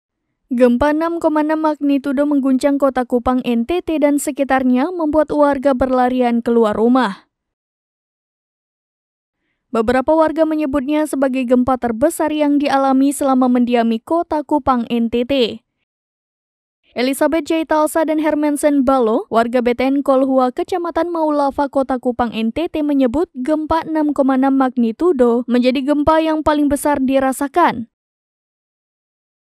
Gempa 6,6 M di Kota Kupang
SERAMBINEWS.COM - Gempa 6,6 magnitudo menguncang kota Kupang, NTT dan sekitarnya membuat warga berlarian ke luar rumah. Beberapa warga menyebutnya sebagai gempa terbesar yang dialami selama mendiami kota Kupang, NTT.

Elisabeth Djaitalsa dan Hermensen Ballo, warga BTN Kolhua, Kecamatan Maulafa, Kota Kupang, NTT menyebut gempa 6.6 magnitudo menjadi gempa yang paling besar dirasakan. Sementara Vinsen Repu, warga Ukitau Kota Kupang mengatakan, gempa ini terbesar selama puluhan tahun berada di Kota Kupang.